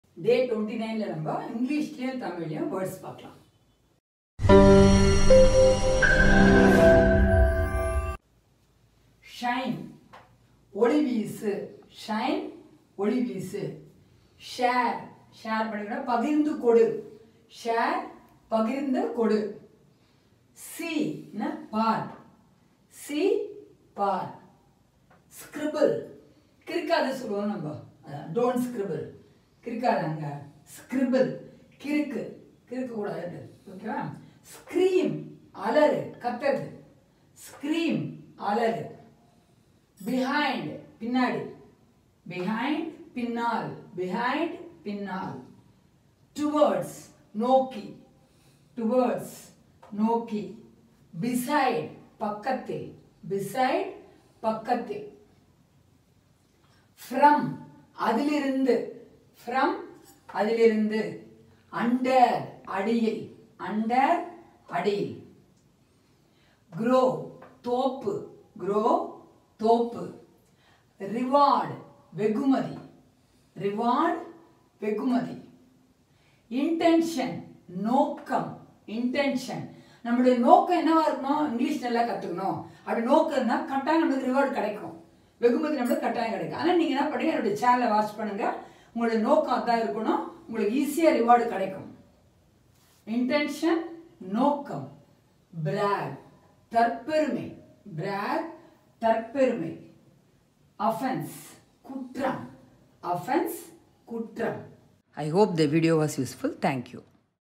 Learn Tamil and English in 30 days | Words Day 29
Day 29 Learn english in tamil
Daily speaking words in english

learn English through Tamil from this video. watch this video and learn to speak English language fluently. daily English words for you.

In this video Daily routine usage words English in Tamil

Watch this video  and support